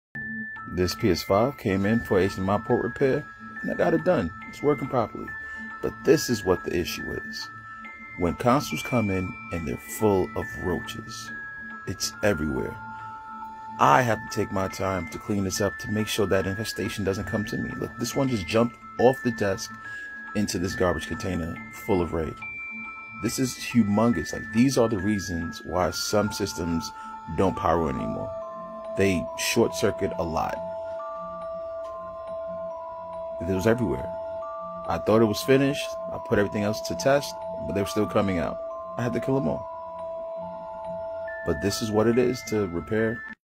The Shocking Truth About Roach Infested PS5s
Here's a shocking trust about roach infestation especially with your PS5 game system:

Cleaning a PS5 game system that has been infested by roaches takes a long time to clean out.

Sometimes when you think you got all of the roaches, there still might be some that are hidden away within the console.

When cockroaches infest gaming consoles, the internal power supply can fry them into the system's components, which can cause the console to malfunction and keep it from turning on.

To keep this from happening to your PS5 or any game system, be sure to keep the area clean around your console, place your console in an elevated area, and make sure to clean your game system regularly.